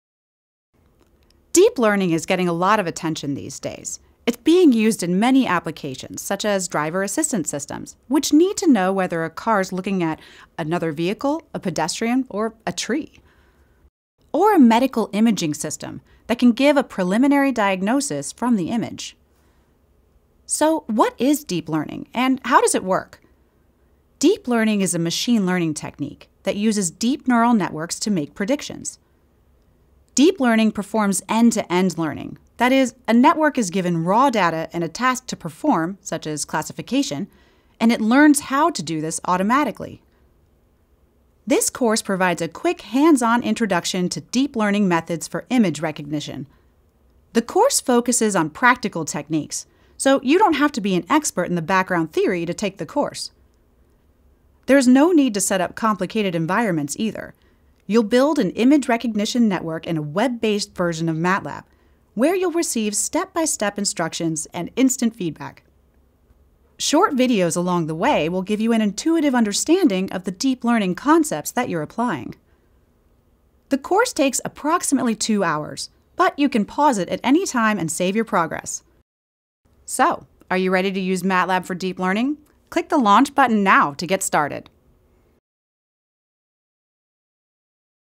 Deep Learning Onramp Overview
Get started quickly with using deep learning techniques in MATLAB® for image recognition. This self-paced, interactive deep learning tutorial includes videos and hands-on exercises with automated assessments and feedback. Topics covered include using pretrained networks, managing collections of data, and performing transfer learning.